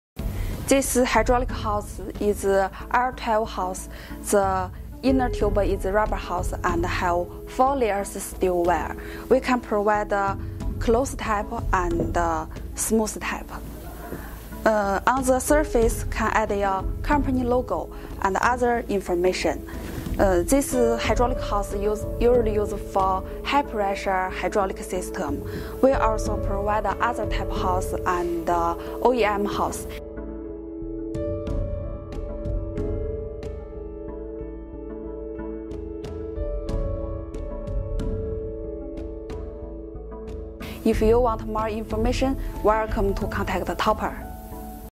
R12 2 high pressure hose ☘️ hydraulic hose manufacturer 3.5 mm high pressure rubber hose m22
Topa's R12 2 high pressure hose is a hydraulic industry standard rubber hose, working pressure range : 175-280 bar. Topa produces hoses for use in a wide range of fluid industries, which features a most common construction for the market requirements.

🌟 High pressure high temperature flexible hose Description:
This Hydraulic hose type is DIN EN856 /SAE100 R12
INNER TUBE: oil resistant synthetic rubber
REINFORCEMENT: four layers wire steel spiral hose
OUTER TUBE: cloth/smooth rubber
Print your logo on the hose
TEMPERATURE RANGE: -40ºC - +100ºC
Working pressure: 175-280bar
Sizes: 3/8" –2 " ID

🌟 High pressure coil hose Feature and benefit:
🎵Provide consistency of performance.
🎵Strict international quality specifications.
🎵Universal hose design manufactured to international specifications.
🎵Increased working pressures for improved reliability and expanded applications.

🌟 Powerjet high pressure washer Application Fields:
For high pressure hydraulic systems in industry and engineering construction.

00:00  50 ft high pressure hose show
00:10  High pressure hose m22 Product show
00:30  Hydraulic hose white label workshop
00:35  Hydraulic hose manufacturer workshop